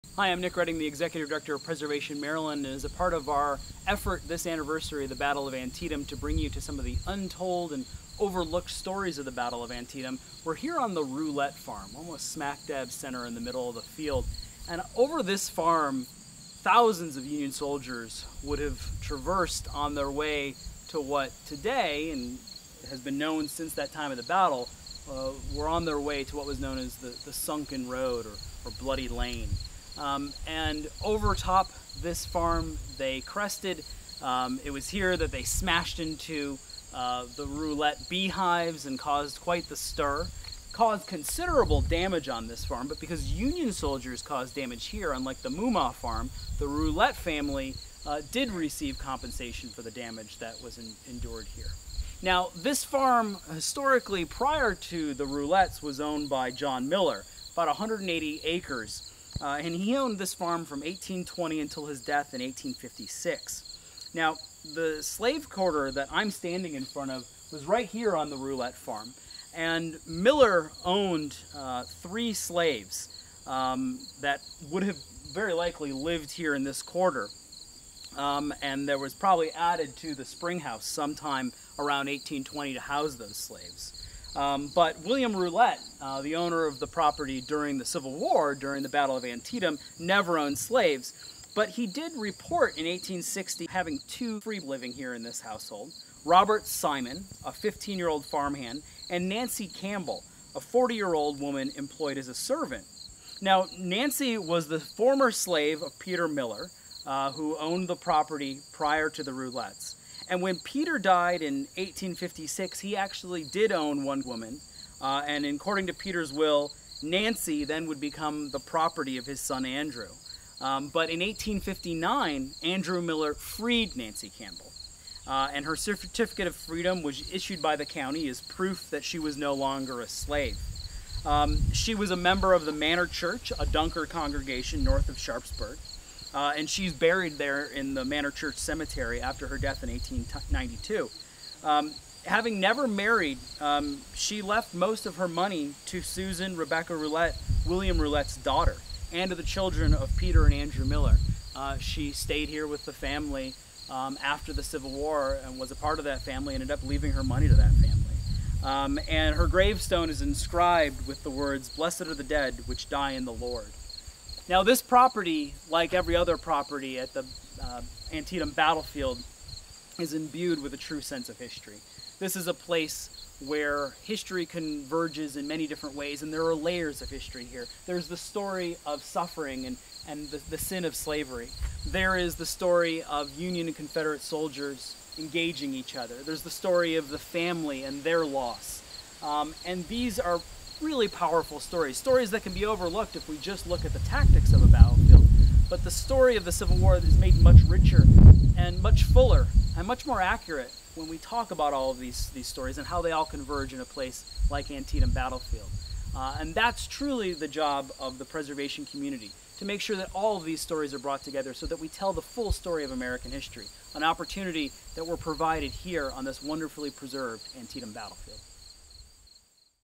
Layers of History at Antietam: Slavery & Freedom at Roulette Farm
ROULETTE FARM

As America confronts the history and legacy of its Civil War, Preservation Maryland Executive Director Nicholas Redding recently visited the Antietam Battlefield, located in Sharpsburg, Maryland, to chronicle just a few of the site's untold stories and unique layers of history.

In this short introduction to the layers of history at the Antietam Battlefield, viewers will explore the power of Antietam's full story and the value of a preserved landscape.

Redding walks up the farm lane from the Mumma Farm to the Roulette Farm, standing at the center of what would become the battlefield itself. Here again, slavery, freedom, and conflict are rooted in the preserved landscape.